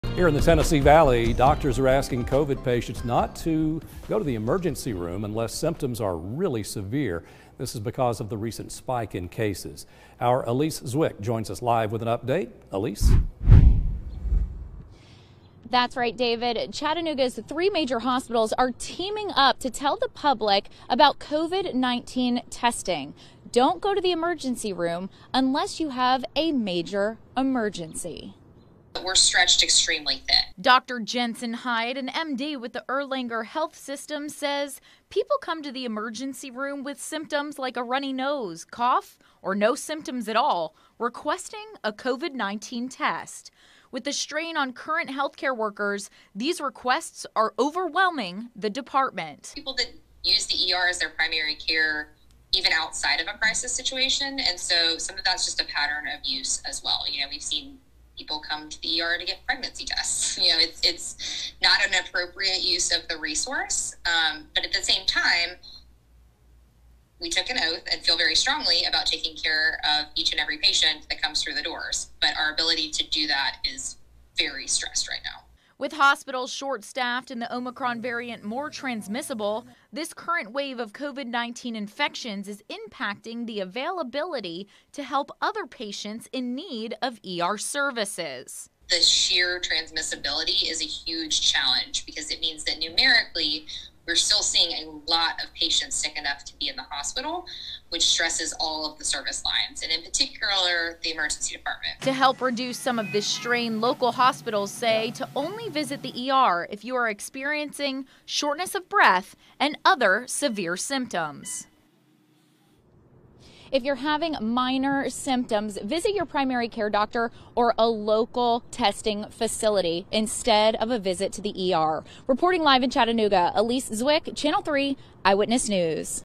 Chattanooga's three major hospitals teaming up for new Covid-19 testing protocol
If you don't have a true emergency, see your primary care doctor.